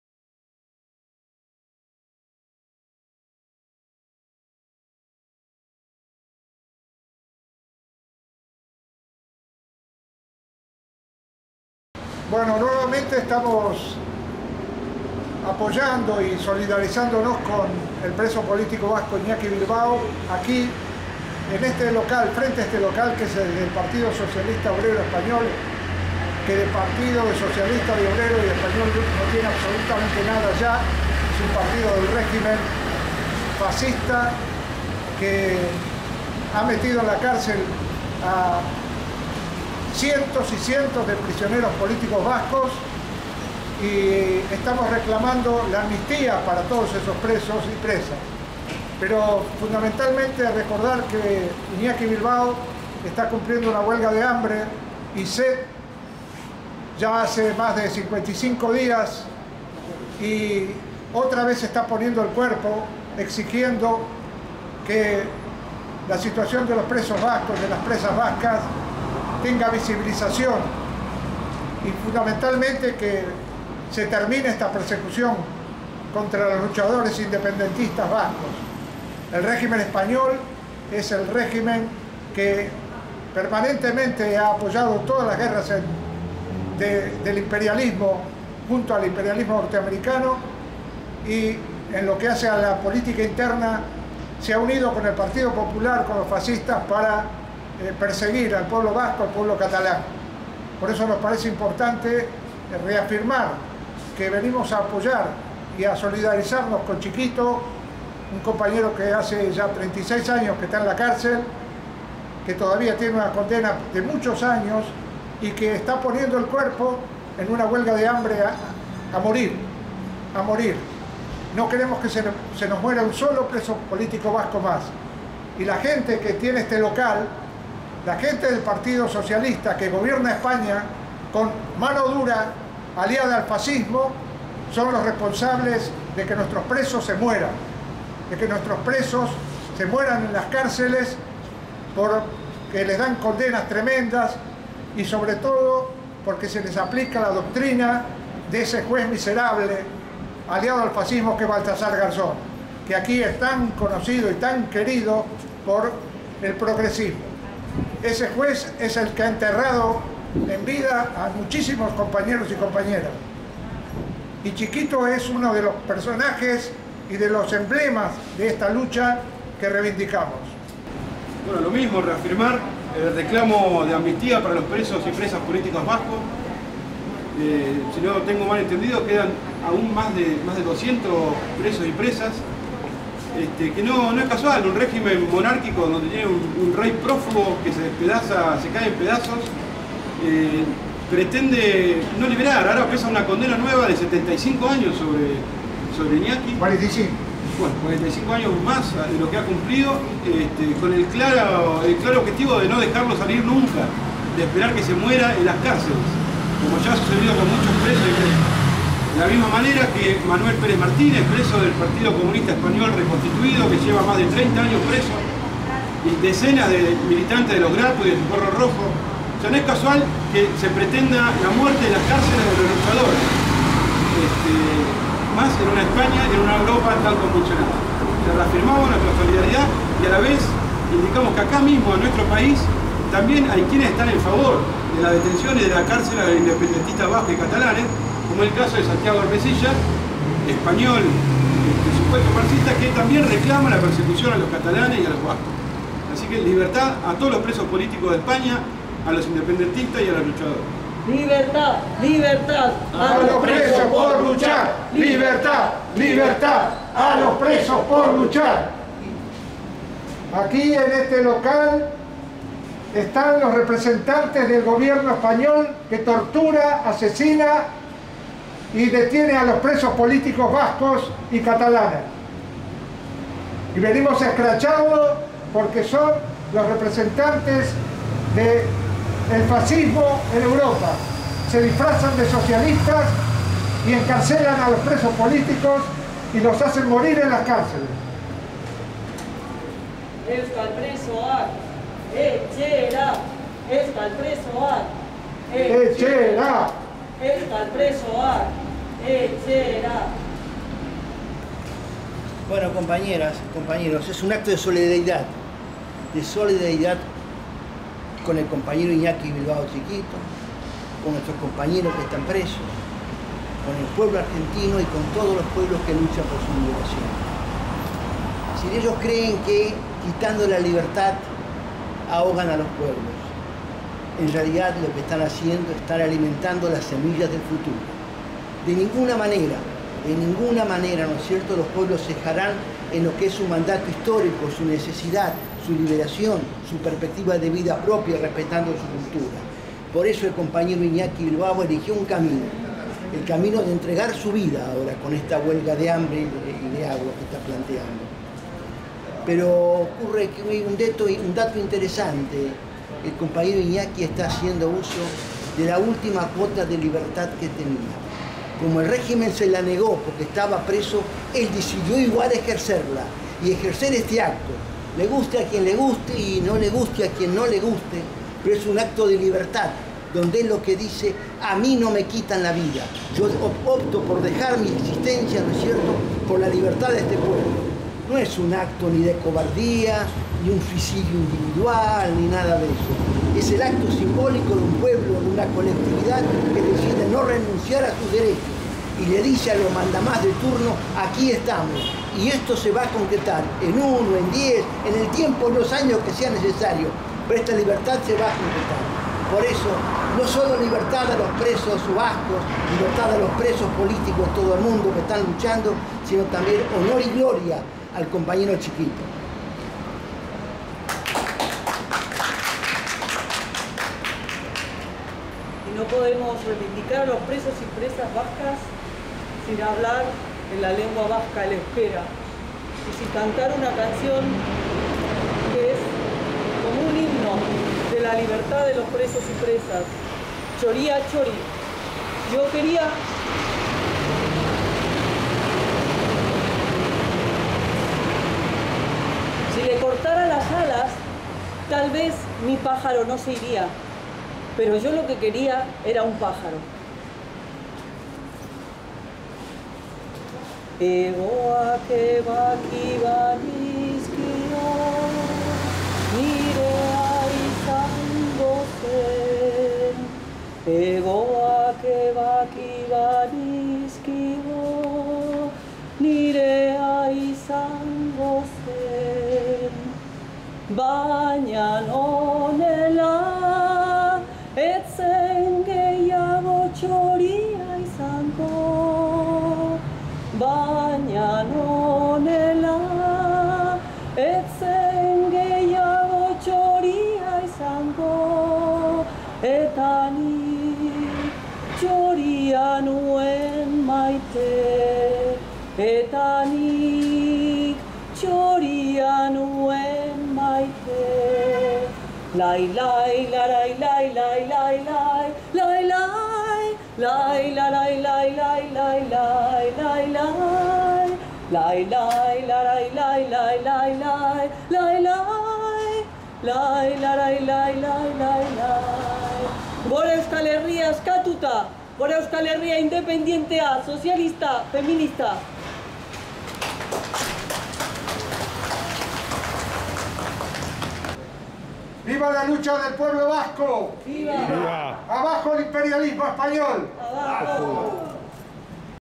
Argentina. Se solidarizaron con Txikito, preso vasco en huelga de hambre
Buenos Aires. Recordaron que el preso vasco Iñaki Bilbao, Txikito, continua la huelga de hambre y sed. Libertad a todxs lxs presxs vascxs.